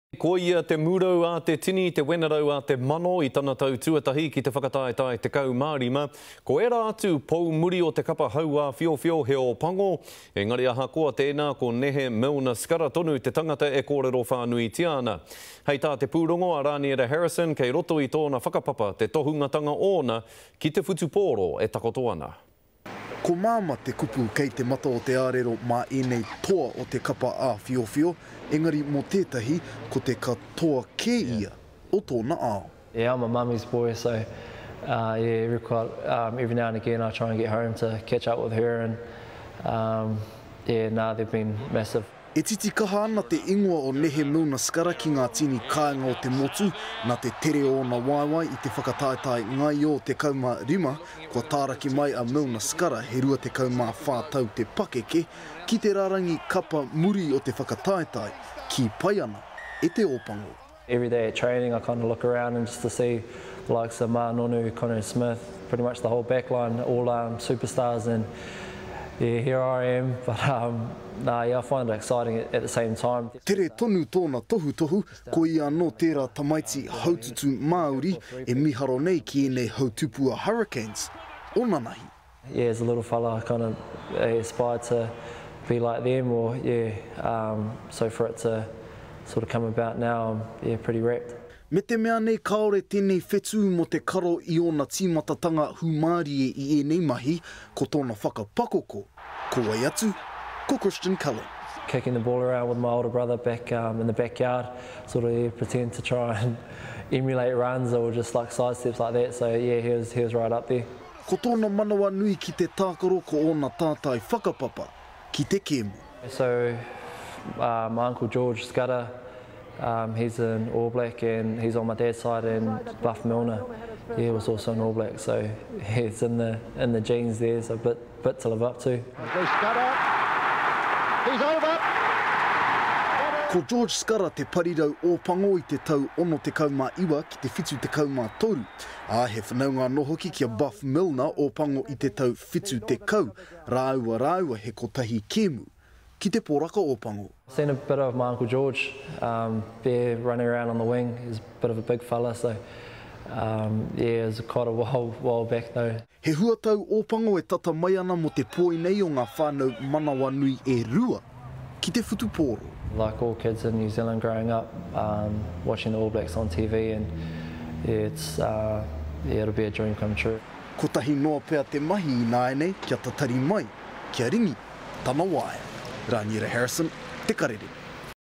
Rugby: The rise of Nehe Milner-Skudder
Nehe Milner-Skudder is having a whirlwind debut Super 15 season. He’s the only non-All Black in the Hurricanes backline but he's impressed in every fixture for the competition leader.